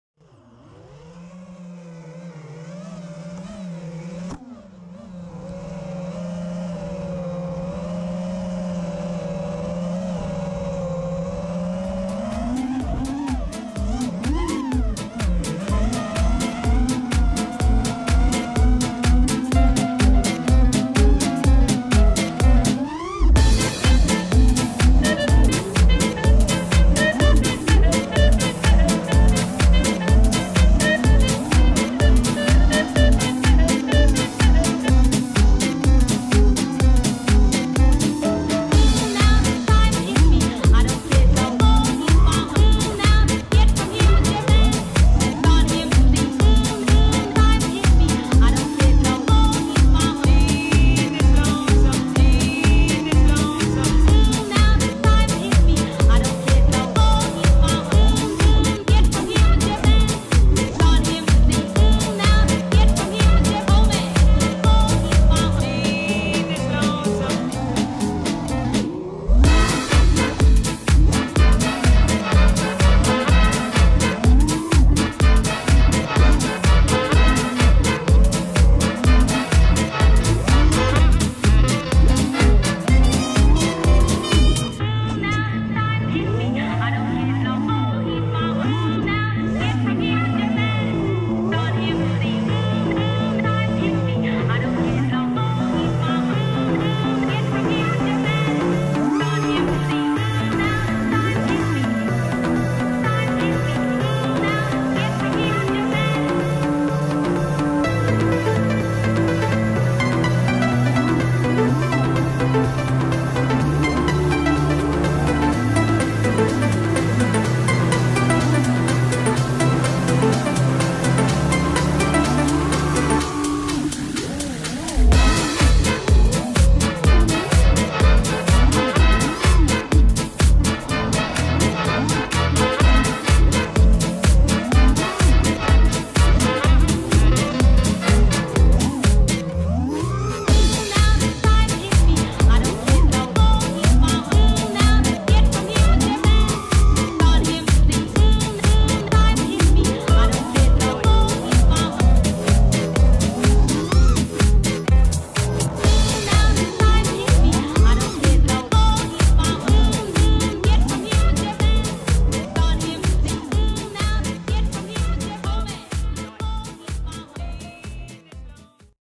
FPV Drone and Abandoned Shopping Mall
Sunset time, 2 charged bateries and racing drone is all what I need sometimes.